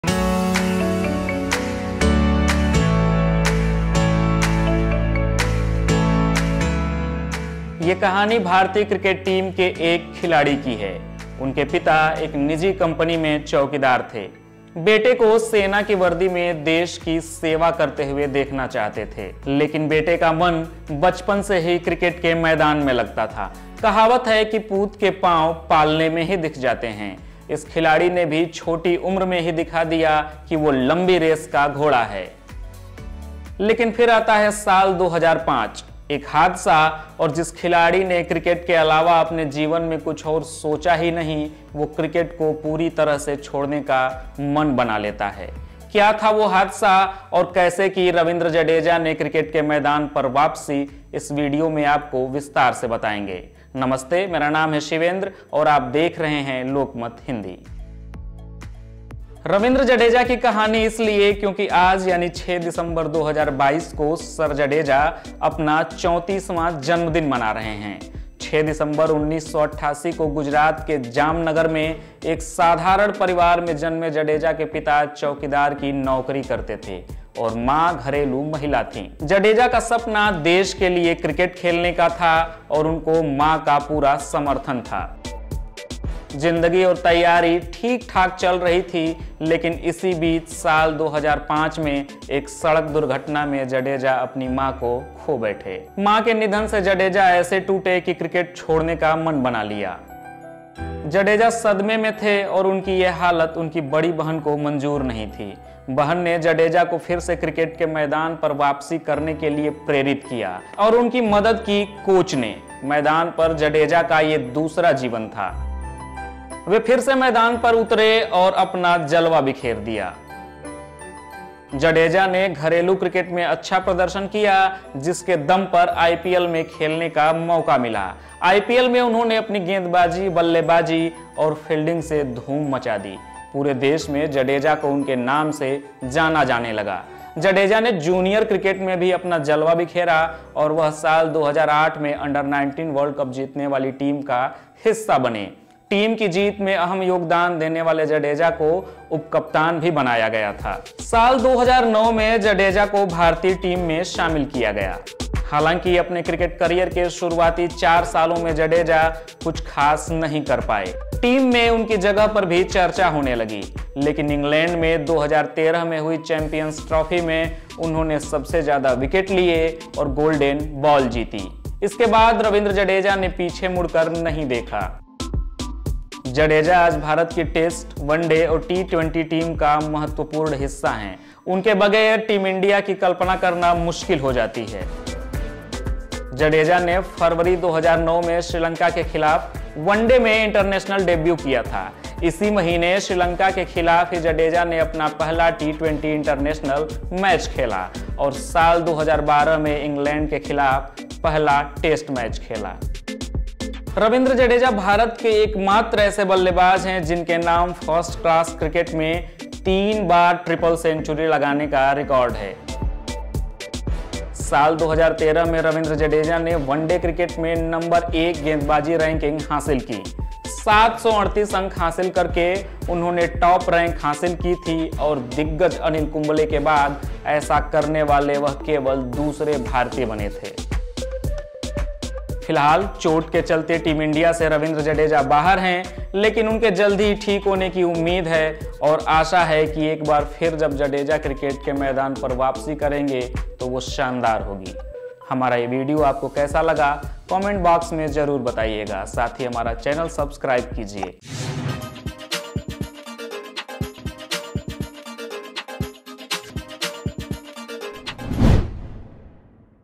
मां की मौत के बाद सदमें में थे Ravindra Jadeja, जानिए कैसे की वापसी | BCCI
विंद्र जडेजा (Ravindra Jadeja) की कहानी इसलिए क्योंकि आज यानी 6 दिसंबर 2022 को सर जडेजा अपना 34वां जन्म दिन मना रहे हैं। 6 दिसंबर 1988 में गुजरात के जामनगर में एक साधारण परिवार में जन्में जडेजा के पिता चौकीदार की नौकरी करते थे और मां घरेलू महिला थीं। जडेजा का सपना देश के लिए क्रिकेट खेलने का था और उनको मां का पूरा समर्थन था। जिंदगी और तैयारी ठीक ठाक चल रही थी लेकिन इसी बीच साल 2005 में एक दुर्घटना में जडेजा की मां का निधन हो गया। मां के निधन से जडेजा ऐसे टूटे कि क्रिकेट छोड़ने की मन बना लिया। कैसे हुई दोबारा जडेजा की क्रिकेट के मैदान पर वापसी, जानने के लिए देखिए ये वीडियो।